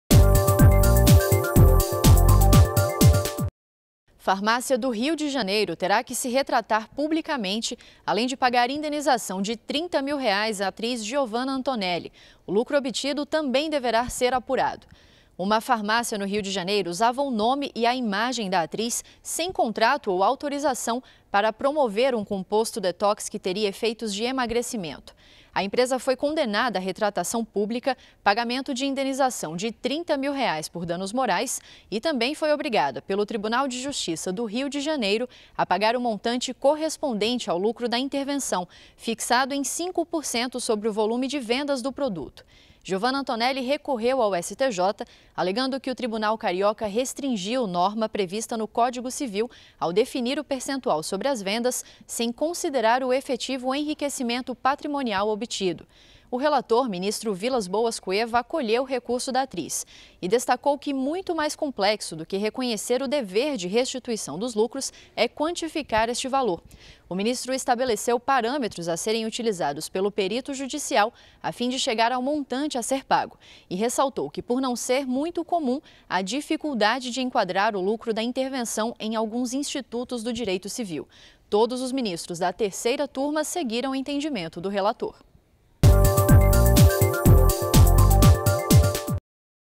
Clique STJ - Farmácia Antonelli (23/10/2018)
Farmácia do Rio de Janeiro terá que se retratar publicamente, além de pagar indenização de 30 mil reais à atriz Giovanna Antonelli.
REsp 1698701